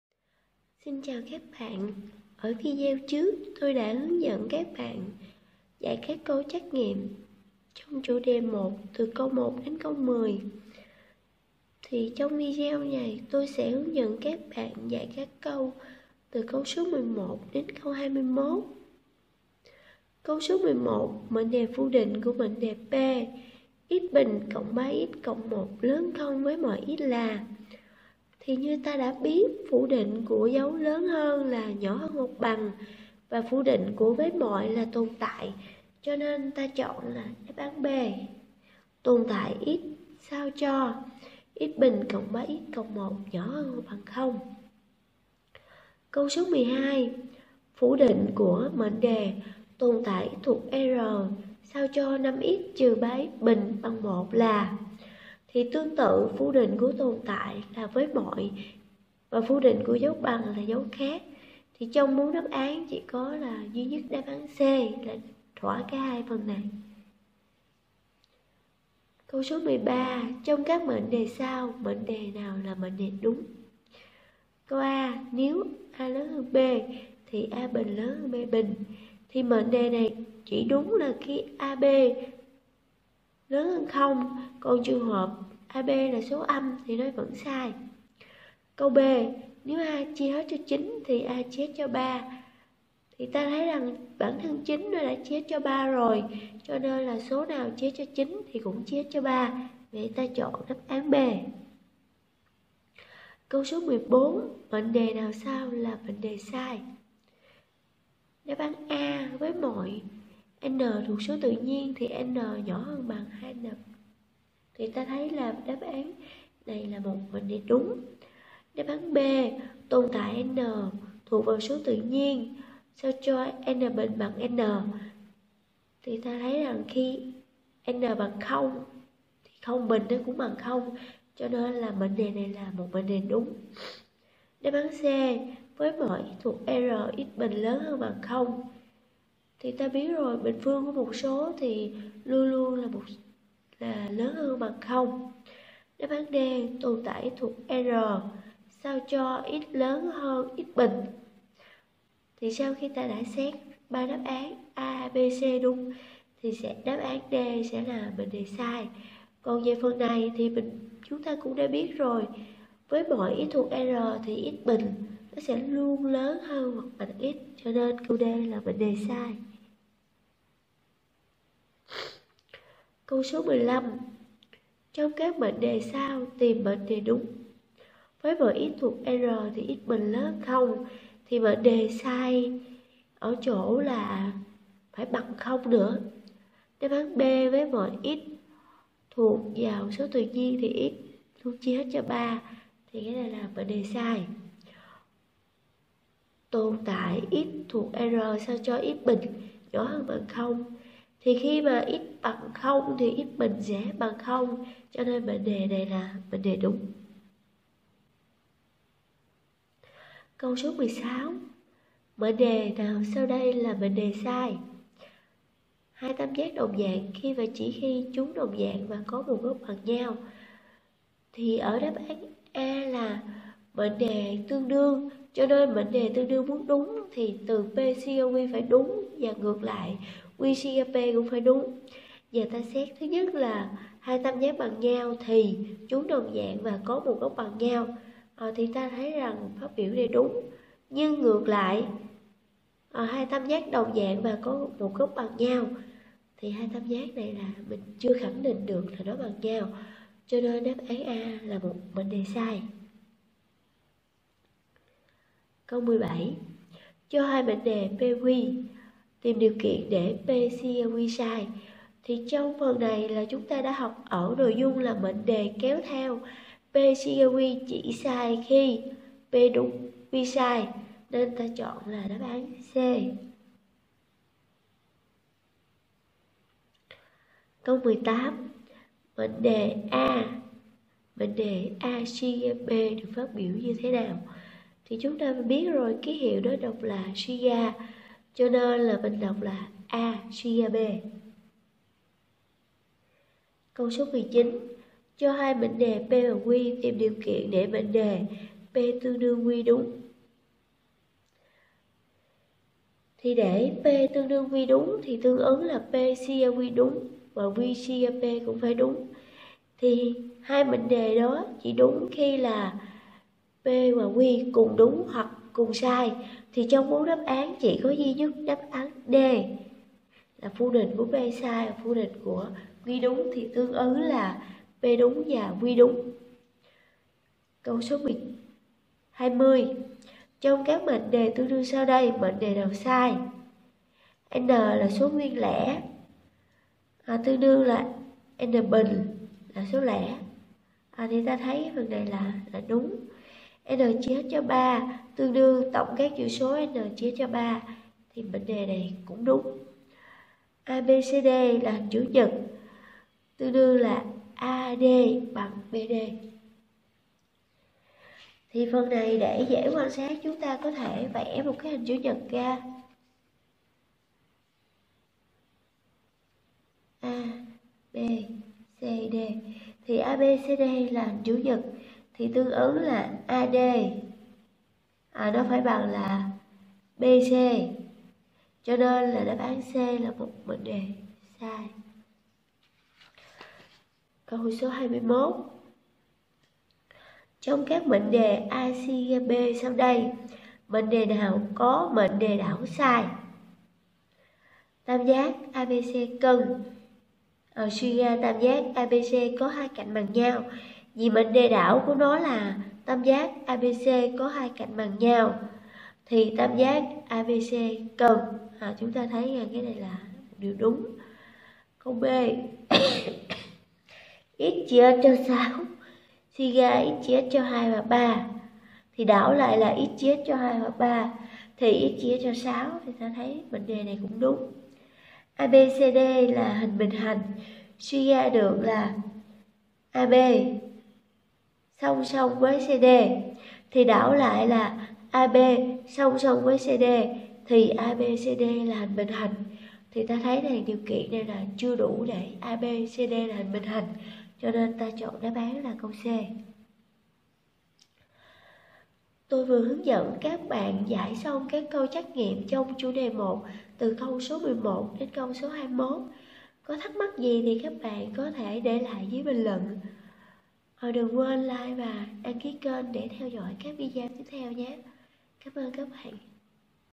Giải đề cương ôn tập môn Toán lớp 10 - Phần 2
Bài tập Đại Số 10
Giải đề Đại số 10 từ cơ bản đến nâng cao. Giải bài tập toán 10, các câu hỏi trắc nghiệm Đại số 10 chuẩn bị kiểm tra học kỳ I. Phần 2
Chúc các em học tôt...